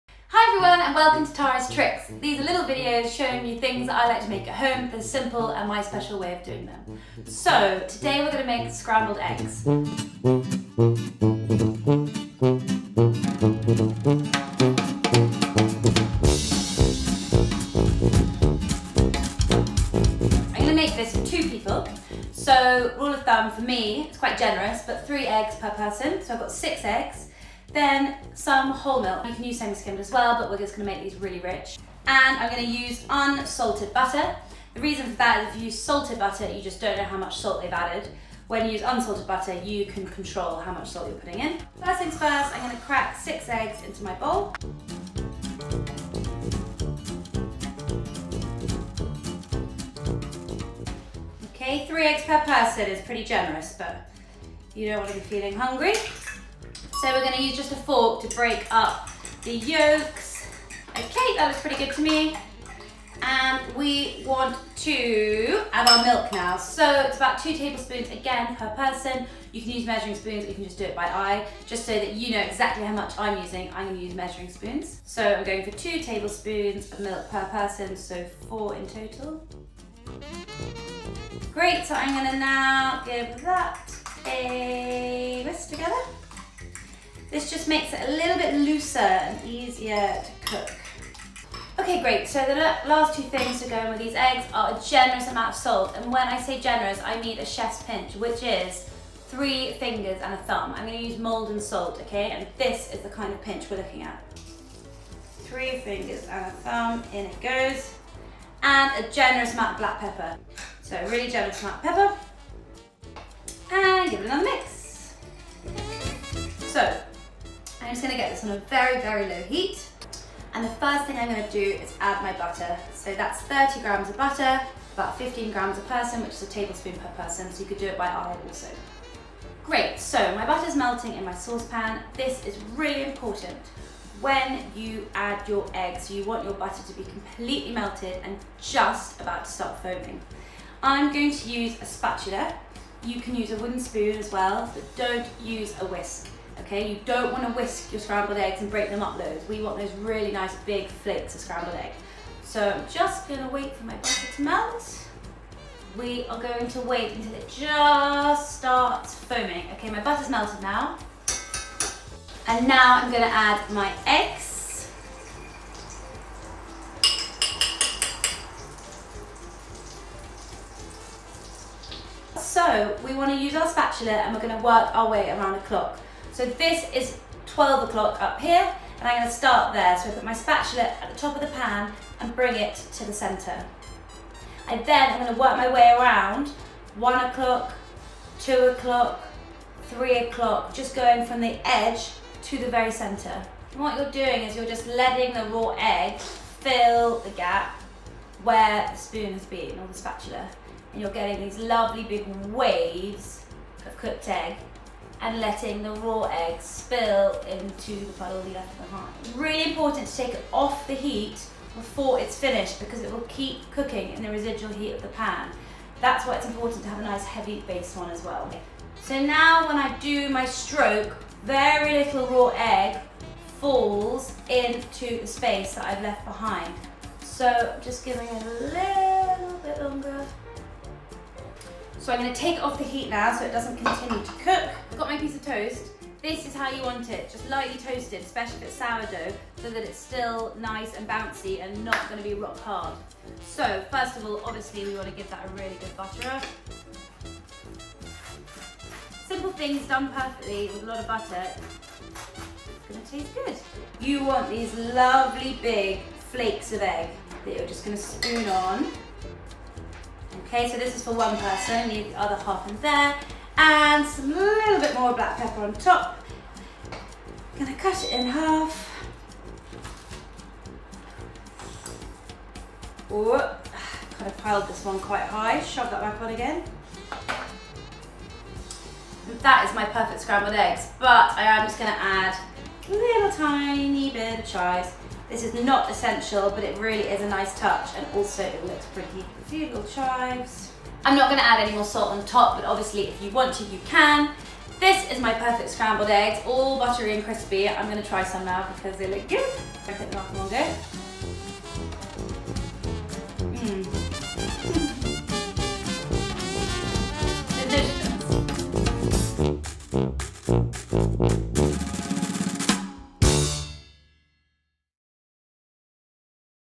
TARA'S TRICKS - Scrambled Eggs
Tara from Tara Cookery reveals her kitchen tricks. This episode demonstrates how to make perfect scrambled eggs.